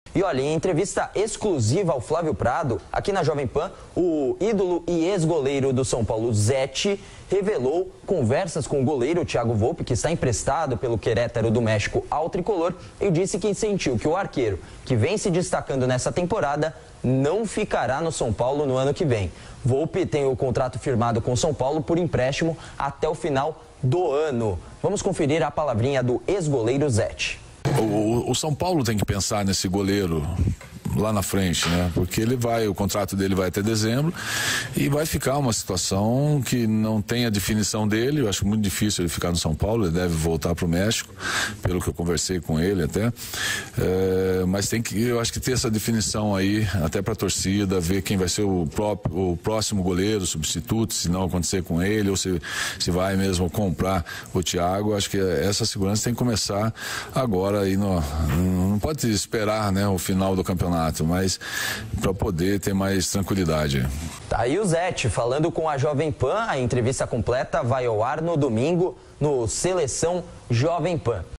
Exclusivo! Zetti TEME saída de destaque do São Paulo!
Zetti deu entrevista exclusiva a Flavio Prado e fez uma importante revelação sobre o futuro de Tiago Volpi no São Paulo.